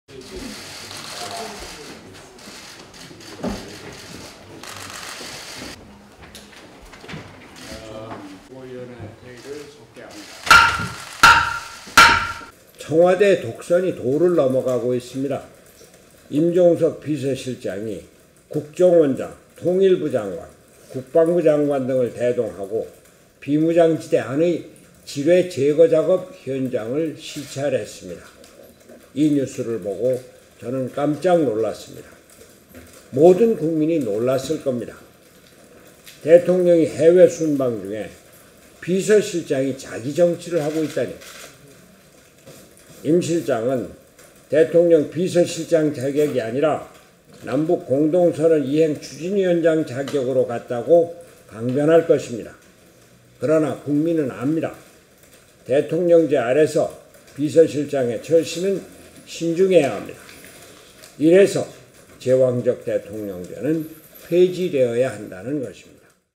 손학규 "임종석, 지뢰제거 시찰…대통령 해외순방 중 자기 정치"
손학규 바른미래당 대표가 19일 국회에서 열린 최고위원회의에서 발언하고 있다.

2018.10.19 일요서울TV 공민식 기자